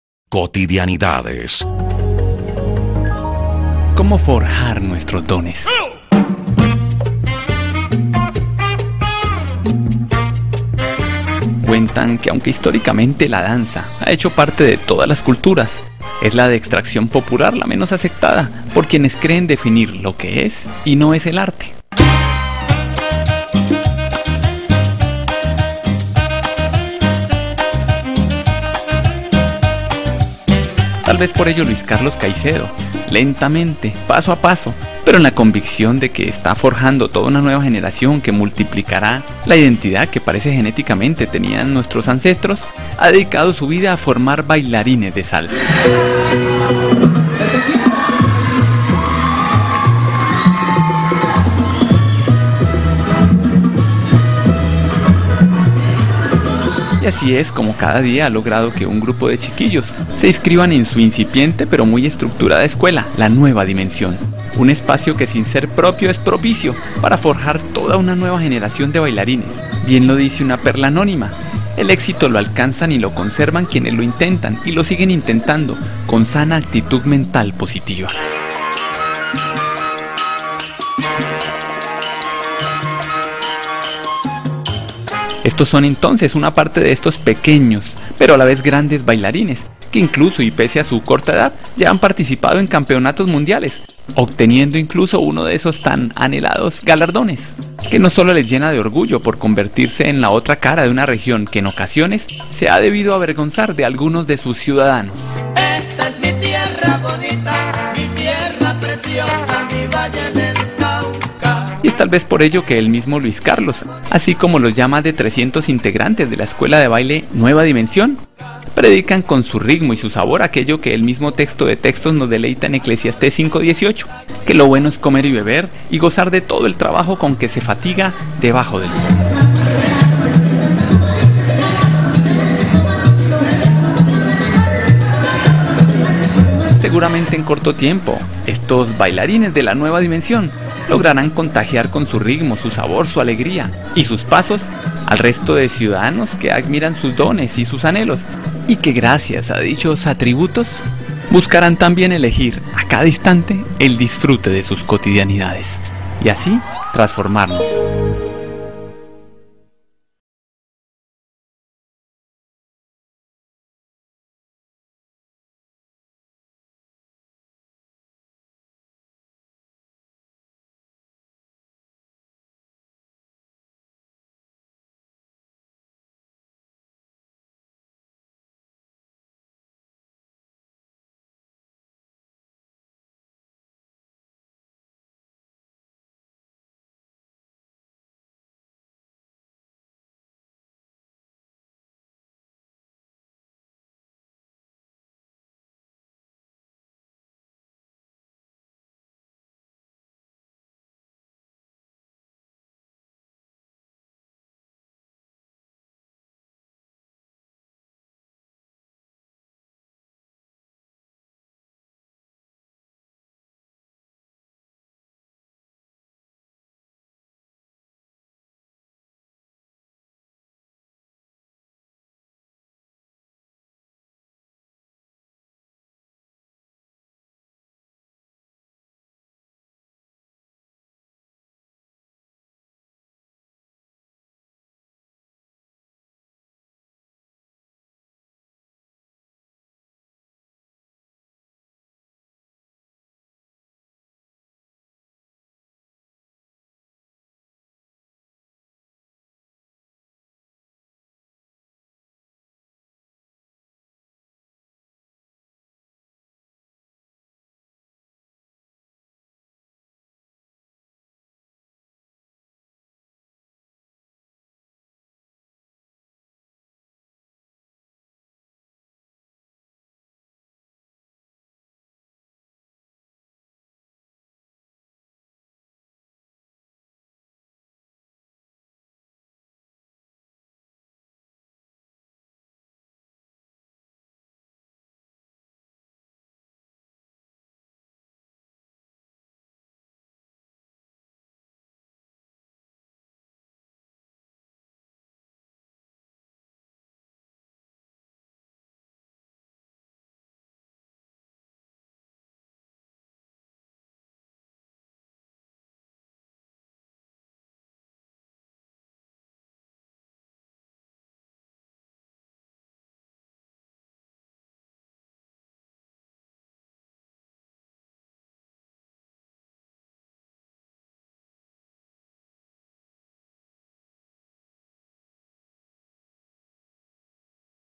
Cotidianidades de una escuela de salsa caleña
Reflexión al respecto de una escuela de salsa caleña - la nueva generación y todo lo que significa para este grupo de jovenes esta cultura popular en sus vidas